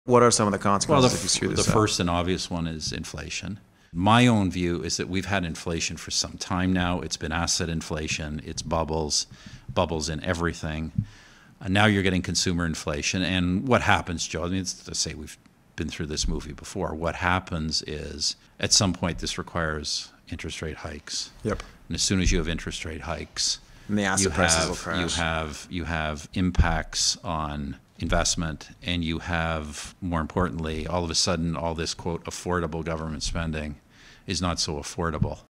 Stephen Harper predicted inflation & interest rate hikes over a year ago
Joe Lonsdale’s full interview with Canadian Prime Minister Stephen Harper from July 2021: